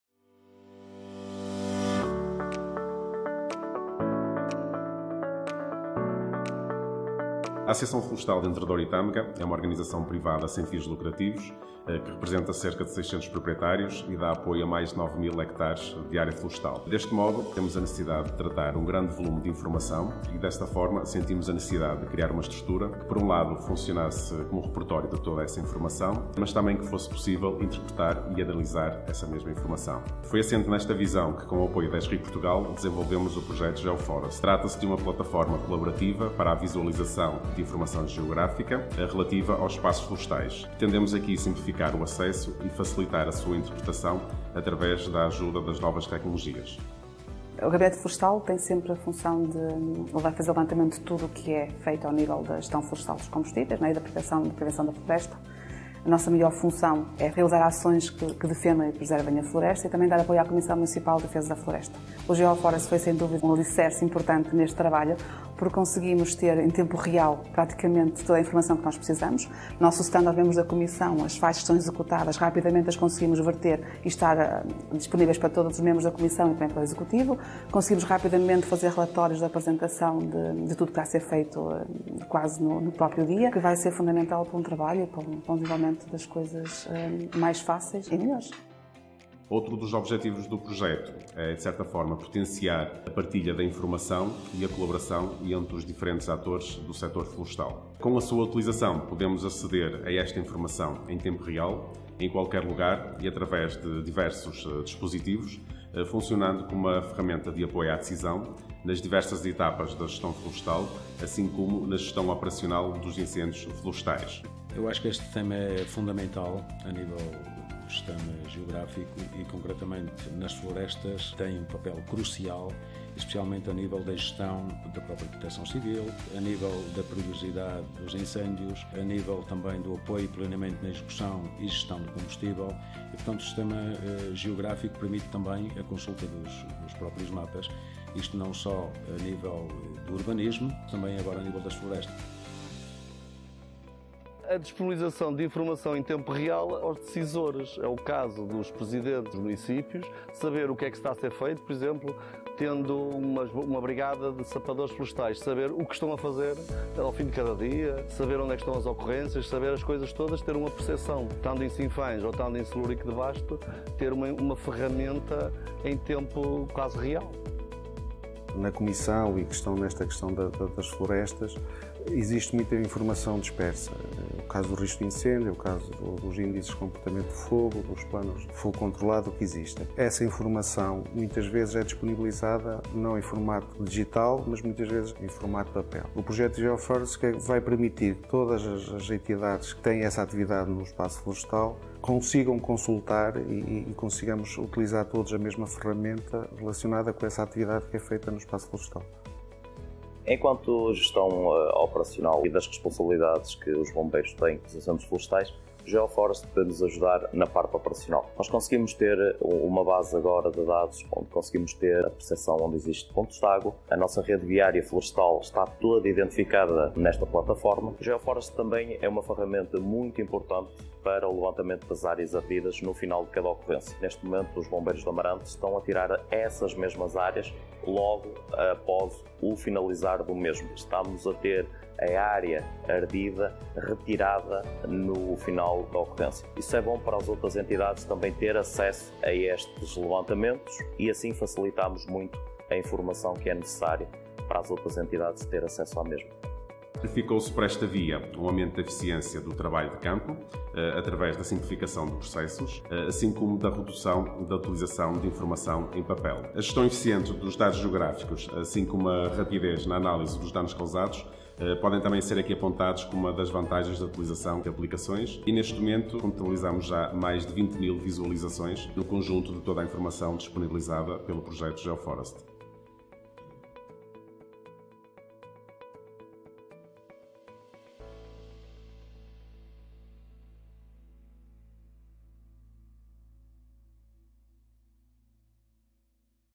Geoforest: A comunidade florestal do Tâmega
📱 Todos os dias, as novas tecnologias ajudam as mais diversas áreas a simplificarem o seu trabalho tornando-o não só mais rápido, como mais eficiente.

🌳 Nesse sentido, fomos até à região do Entre Douro e Tâmega, onde está a primeira Smart Community Florestal, descobrir de que maneira a inclusão da tecnologia Esri, através da aplicação Geoforest, veio melhorar a forma como o trabalho em torno do espaço florestal é realizado nos vários postos de intervenção!